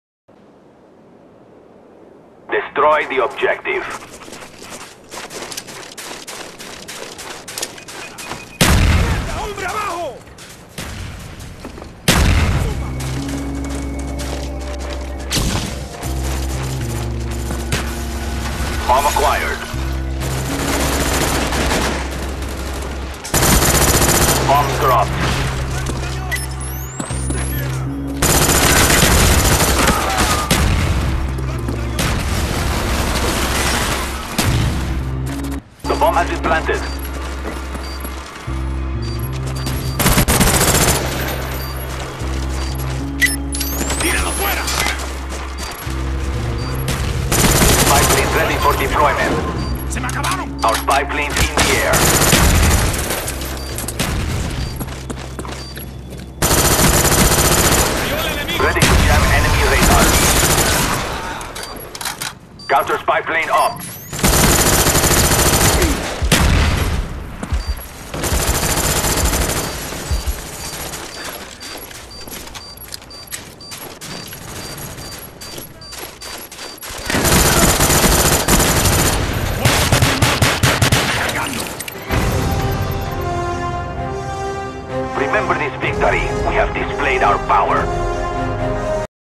Sabotage Team Playing part 1
Just a quick 2 minute sabotage destroying/defending the objective.